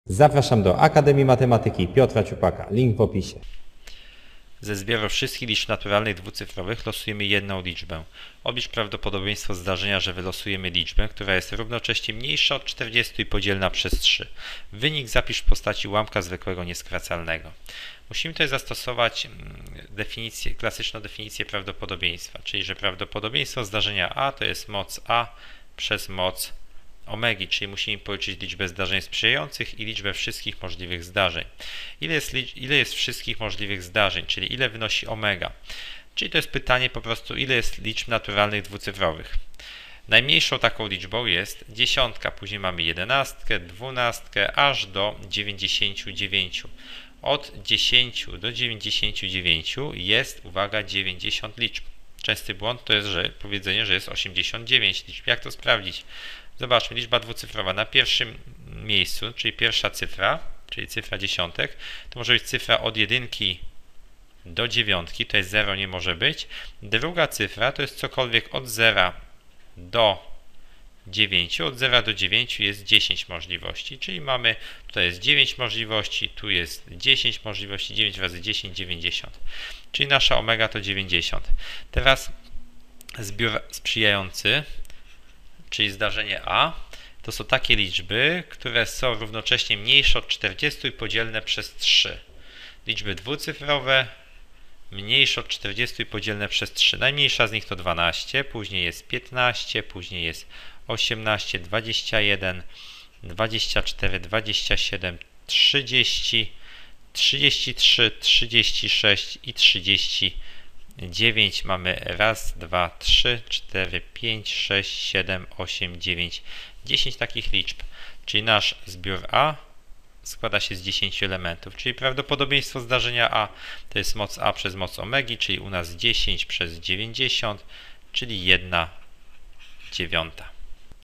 Matura maj 2017 zadanie 33 Ze zbioru wszystkich liczb naturalnych dwucyfrowych losujemy jedną liczbę
Ze zbioru wszystkich liczb naturalnych dwucyfrowych losujemy jedną liczbę. Oblicz prawdopodobieństwo zdarzenia, że wylosujemy liczbę, która jest równocześnie mniejsza od 40 i podzielna przez 3. Wynik podaj w postaci ułamka zwykłego nieskracalnego.